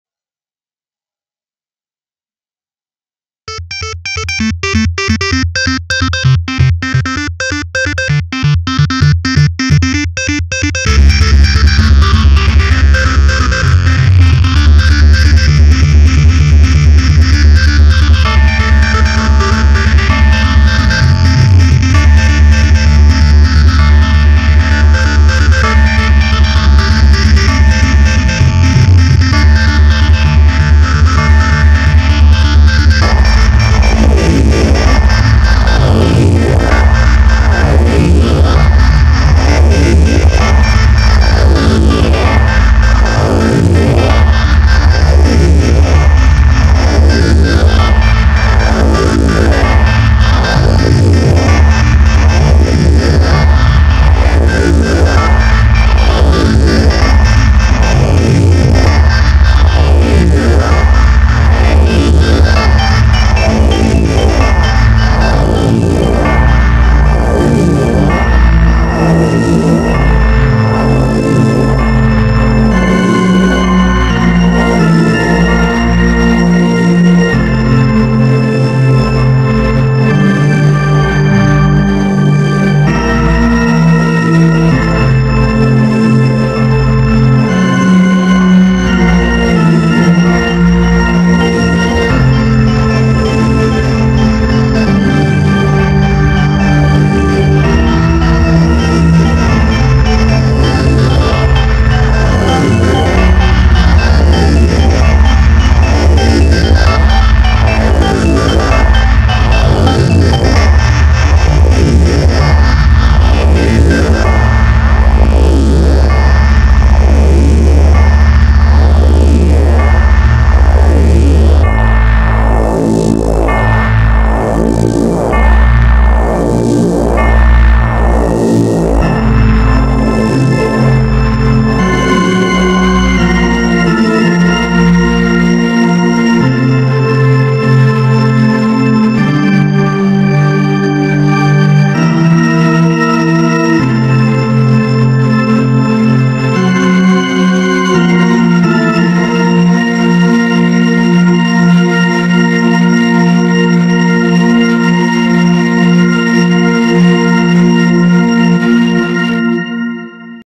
What genre is this?
I mixed organ with "video game menu music"